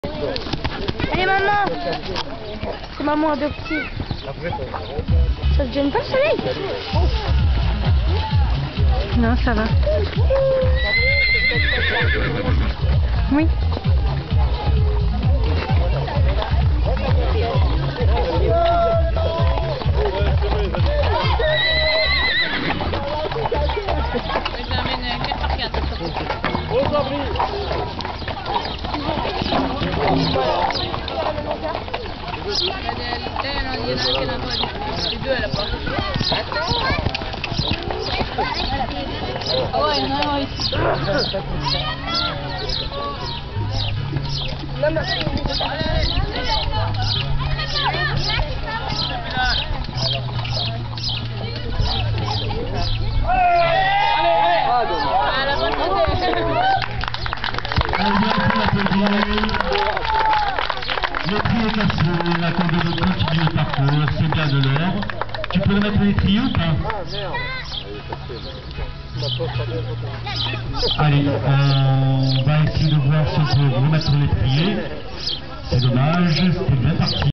CSO 29.06.08 Oxyd et A. Laure 1ère partie...
Première 1m pour Oxyd.
Anne Laure a perdu les étriers en début de parcours et à tenu un temps...